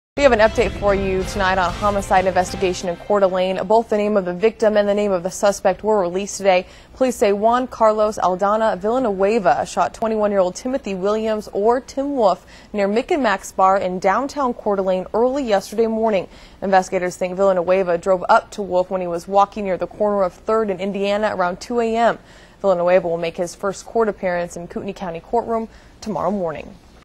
Police identify murder victim in CDA shooting
Coeur d'Alene Police say 21-year-old Timothy Wolfe was killed in a drive by shooting early Saturday morning in Coeur d'Alene. KXLY4's Tori Brunetti has more.